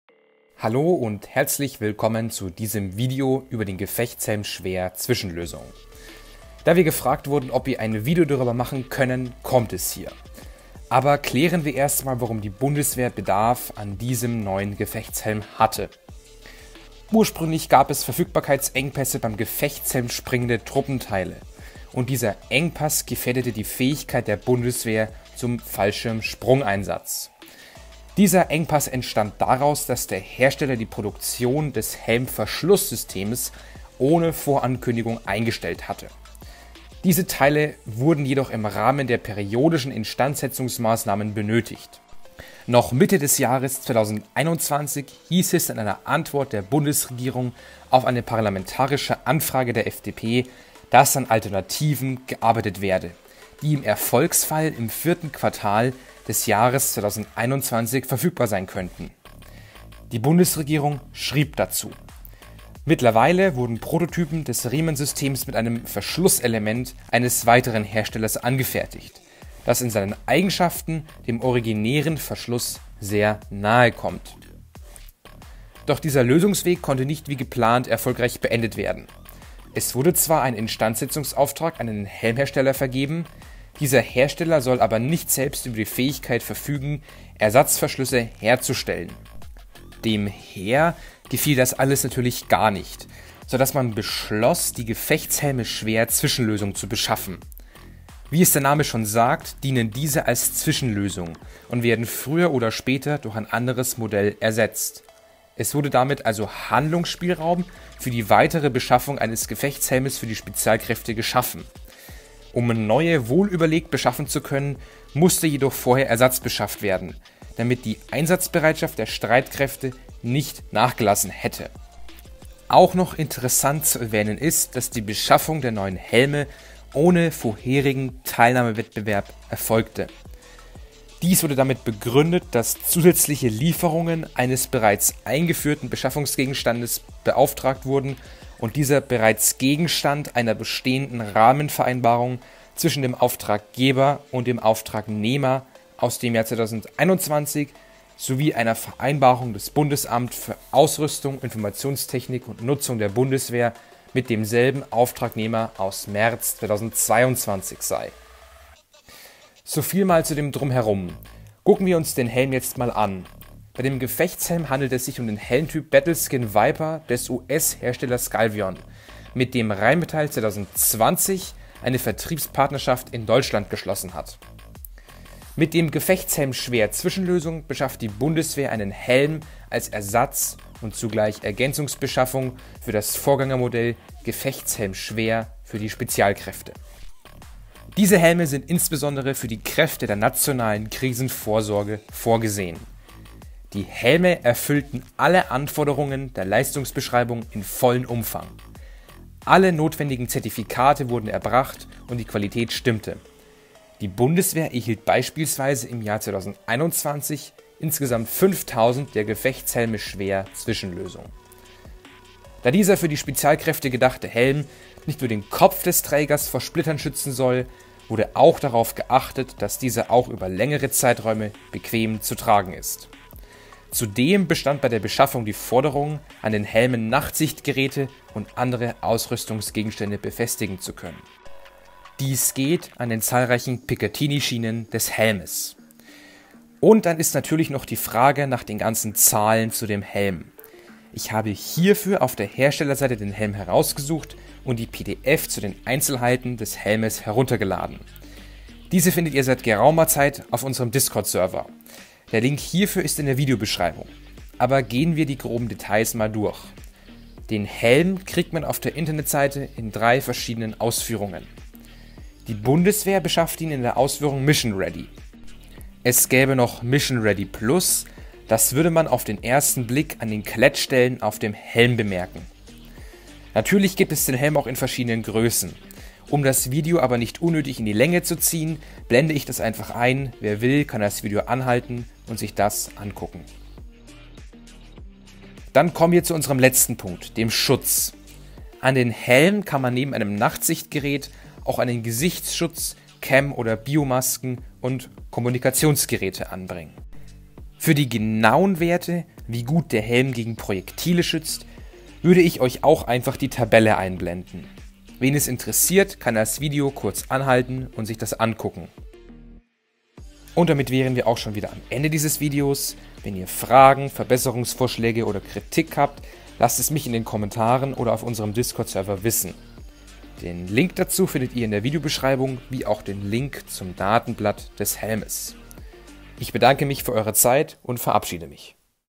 Gefechtshelm schwer Zwischenlösung erklärt -der neue Helm der Spezialkräfte-
Mit dem „Gefechtshelm schwer Zwischenlösung“ beschafft die Bundeswehr einen Helm als Ersatz- und zugleich Ergänzungsbeschaffung für das Vorgängermodell „Gefechtshelm schwer“ für die Spezialkräfte. Diese Helme sind insbesondere für die Kräfte der nationalen Krisenvorsorge (NatKV) vorgesehen. Als Zwischenlösung bis zur Verfügbarkeit des „Gefechtshelms SK (Streitkräfte)“ werden zunächst 5.000 Helme für die Kräfte NatKV beschafft. Um den Ansprüchen an einen Helm gerecht zu werden, sind Zertifikate zum Beispiel zur ballistischen Haltefähigkeit erforderlich. Darüber hinaus muss der Helm einem ausführlicheren Truppenversuch standhalten. Das kostet Zeit, die für die Sicherheit der Soldatinnen und Soldaten aber aufgebracht werden muss.

Keines der als Footage gezeigten Videos gehören uns!